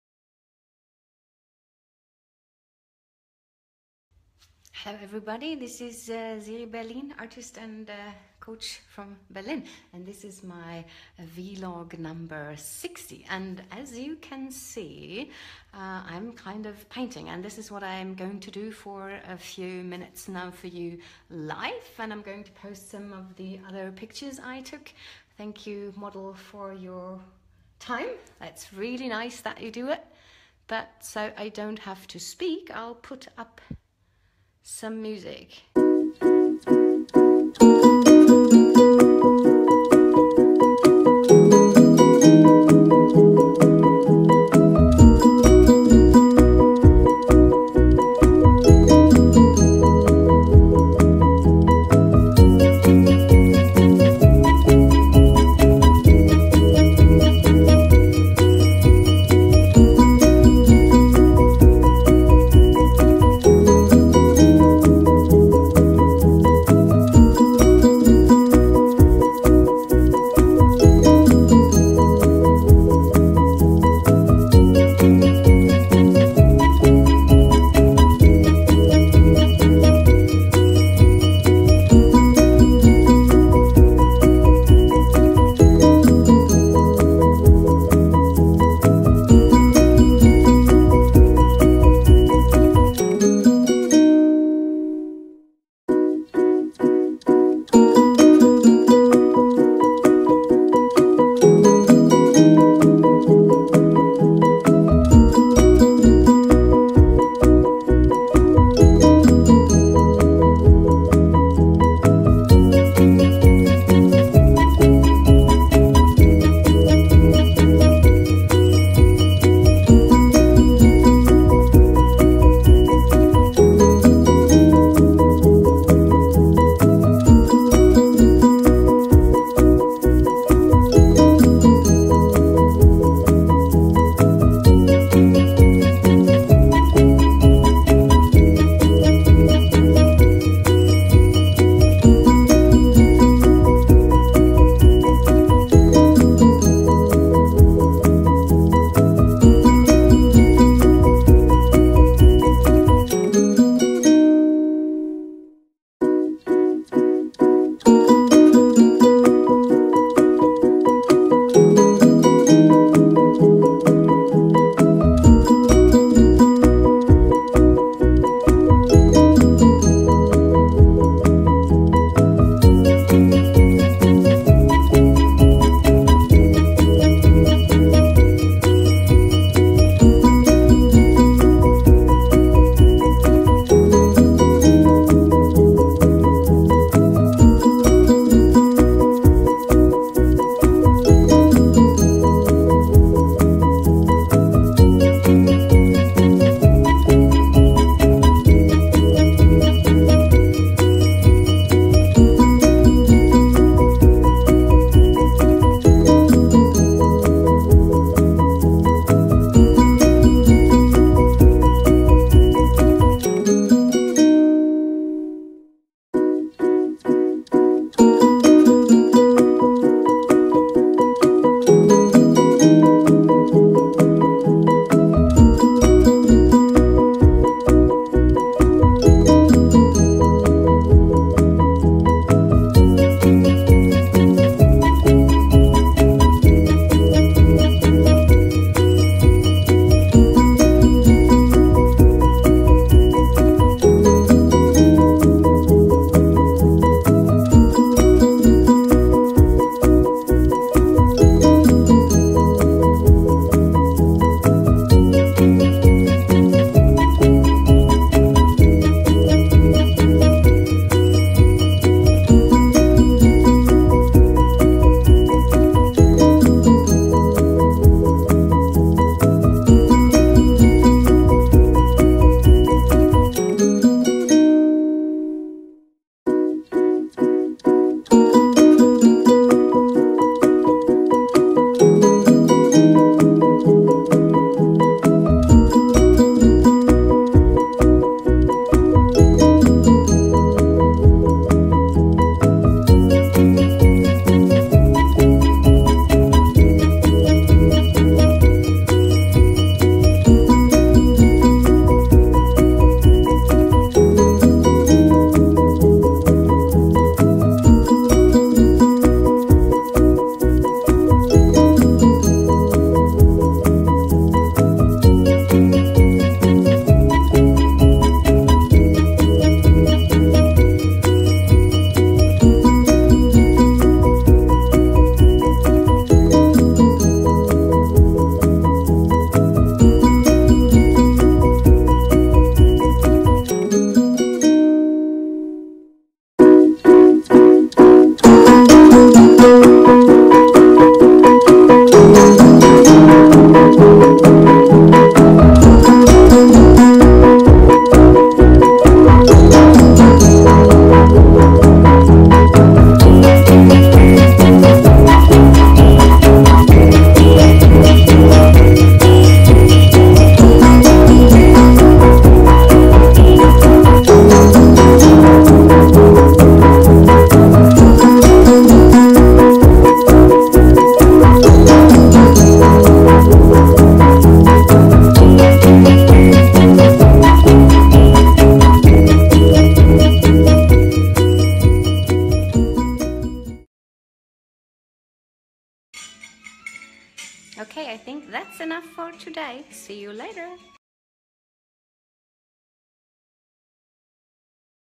Vlog No. 60 - Life Painting
I'm Siri Berlin, artist and coach from Berlin. This is No. 60 of my 99-days-vlog challenge which I do on Facebook. Today I had the luck to paint a beautiful woman from one of my life drawing meetings. The complete painting process as time lapse will be online soon.